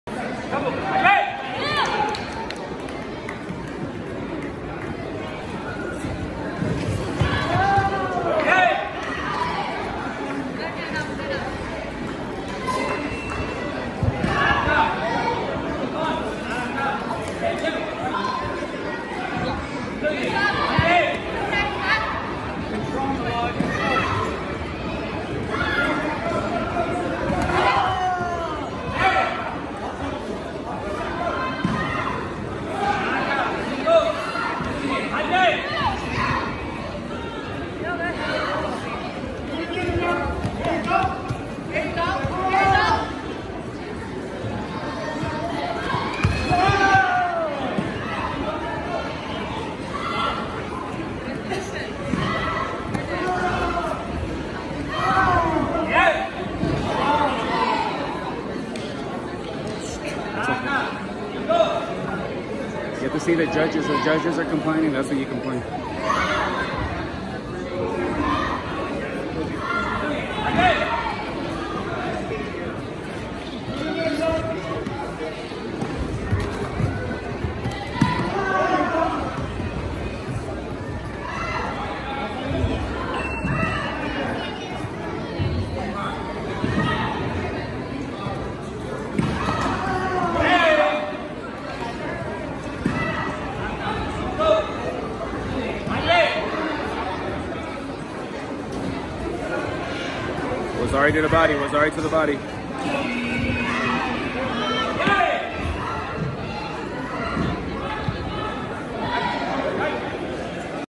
Milad Samadi
2nd Round 12-13 Intermediate 21st Annual Ryukyukan International Karate Tournament